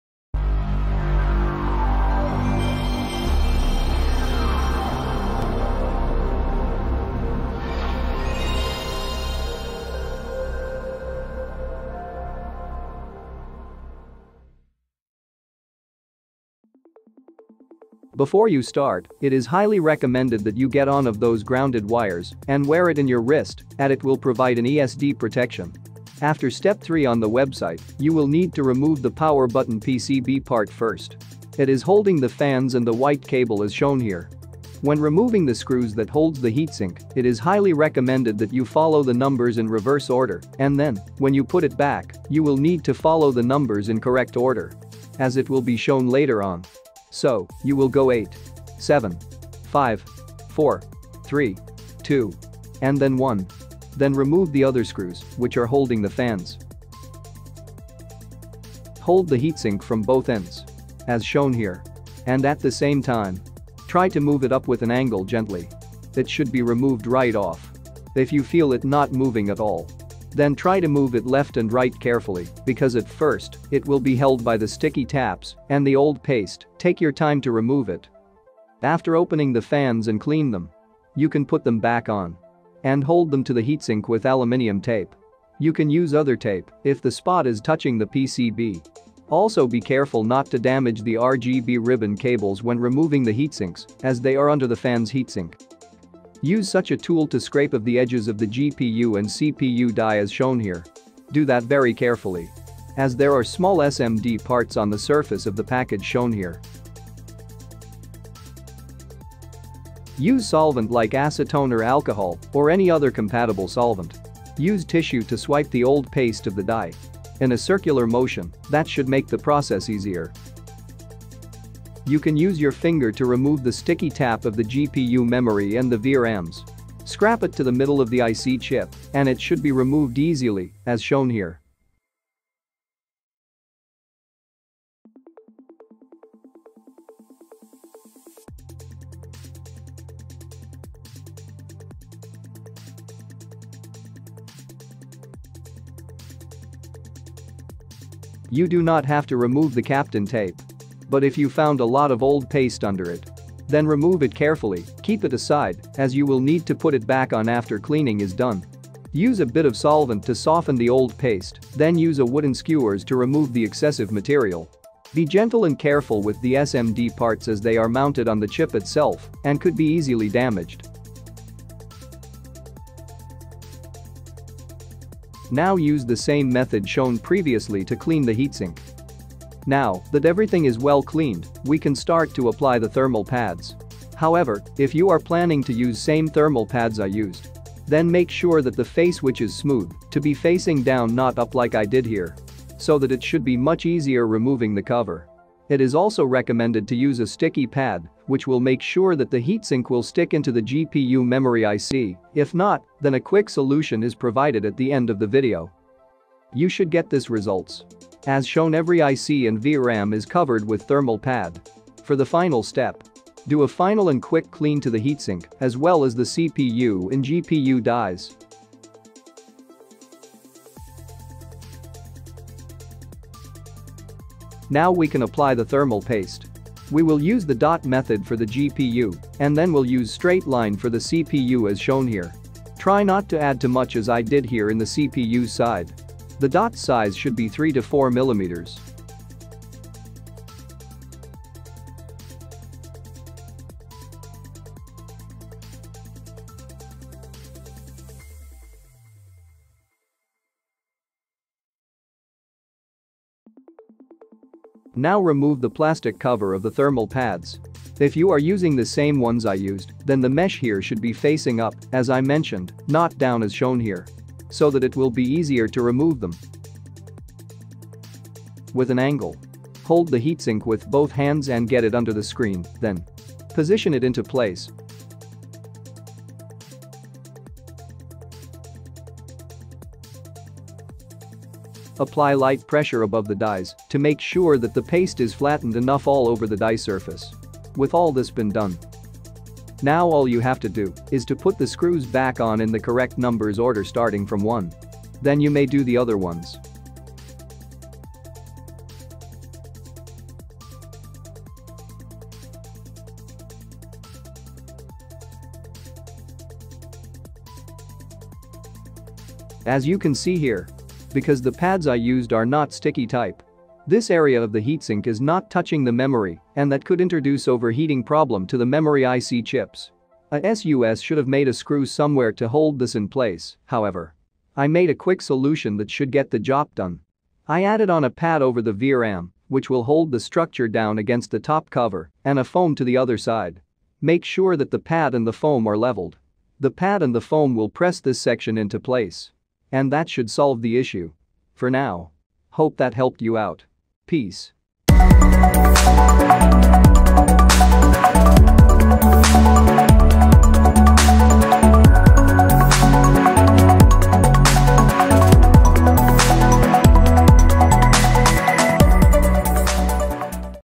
THERMAL PASTE REPLACEMENT FOR ASUS ROG ZEPHYRUS S GX531GW
Since my the website is down, for now please follow ifixit steps until step 5 then follow the Video: